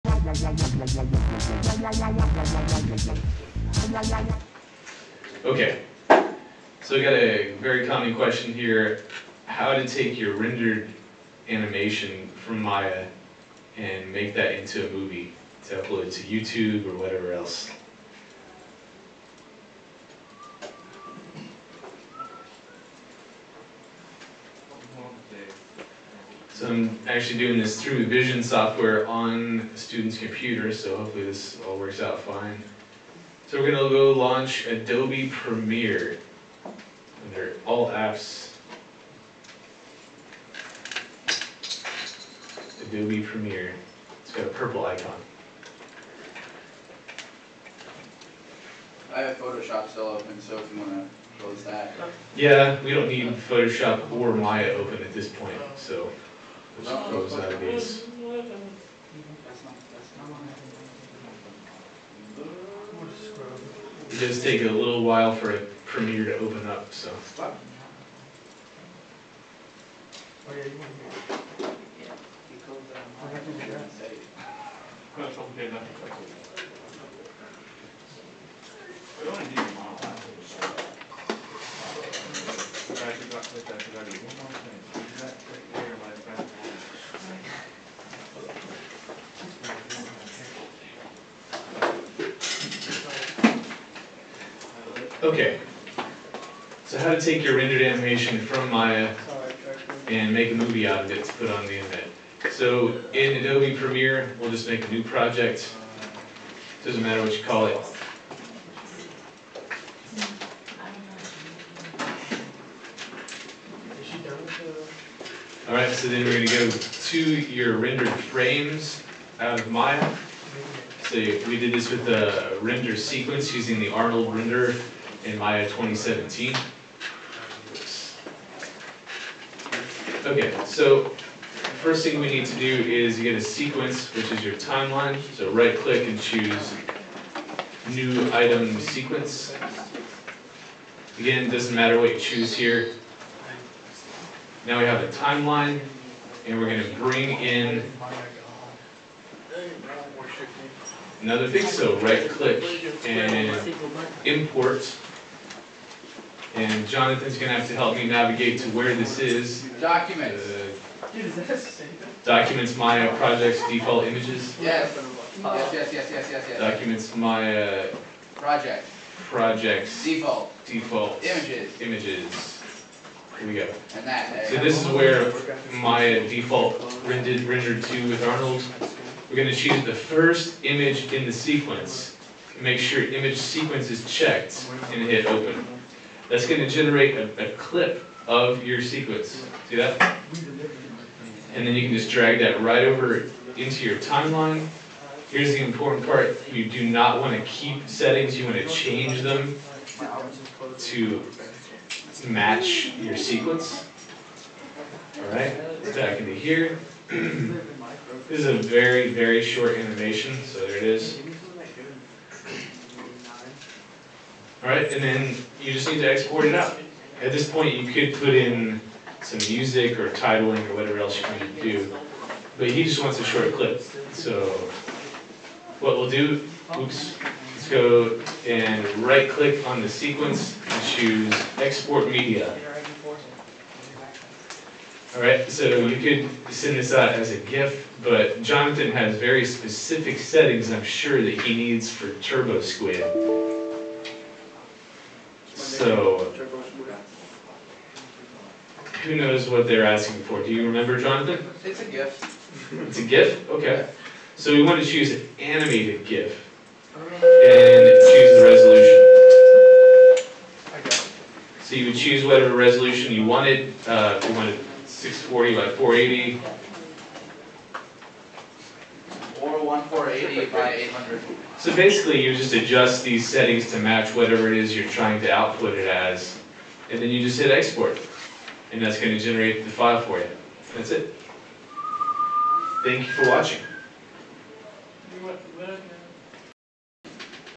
Tutorial: How to turn your image sequence into a video with Adobe Premiere
In this short tutorial I show my students how to take your rendered image sequence from Maya and import that into premiere as an image sequence and then export as a video file.